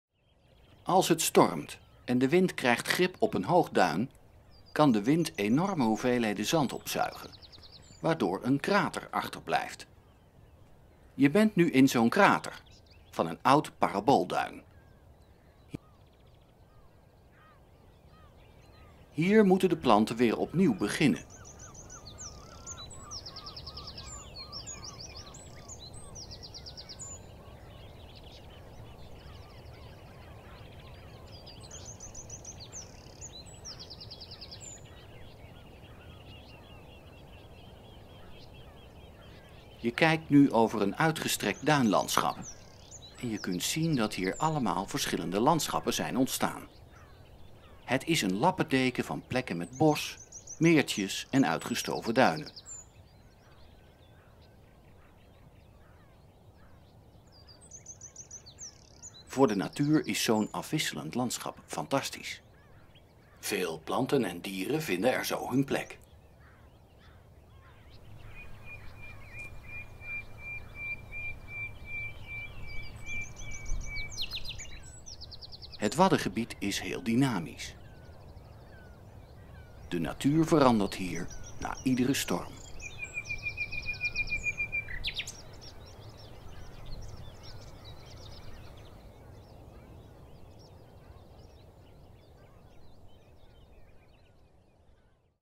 Uitgestorven Duinpan
Als het stormt en de wind krijgt grip op een hoog duin, kan de wind enorme hoeveelheden zand opzuigen waardoor een krater achterblijft.
Je bent nu in zo’n krater, van een oud paraboolduin.
Hier moeten de planten weer opnieuw beginnen.
Je kijkt nu over een uitgestrekt duinlandschap. En je kunt zien dat hier allemaal verschillende landschappen zijn ontstaan.
Het is een lappendeken van plekken met bos, meertjes en uitgestoven duinen.
Voor de natuur is zo’n afwisselend landschap fantastisch.
Veel planten en dieren vinden er zo hun plek.
Het waddengebied is heel dynamisch, de natuur verandert hier na iedere storm.